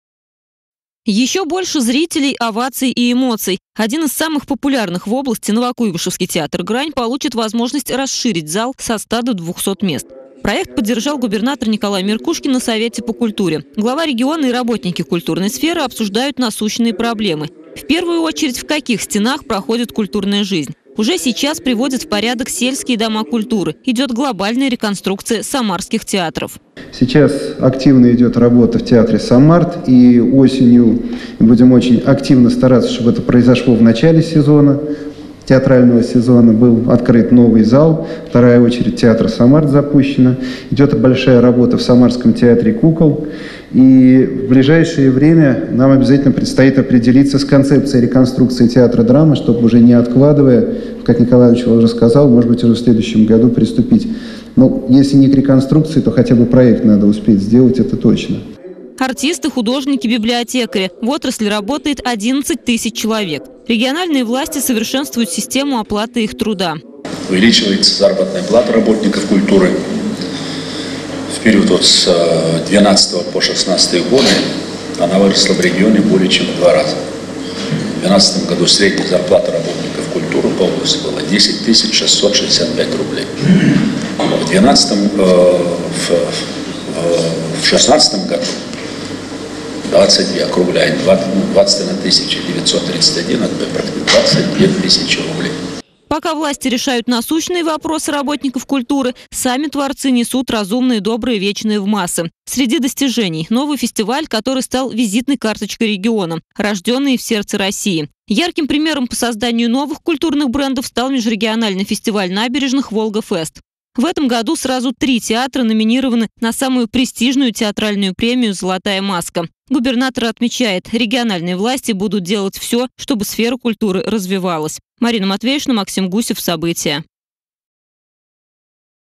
Глава региона и работники культурной сферы обсудили насущные проблемы
Зарплаты работников культуры в Самарской области выросли более чем в два раз за последние четыре года. 63-й - единственный регион в стране, где средний заработок в этой отрасли выше, чем в сфере образования.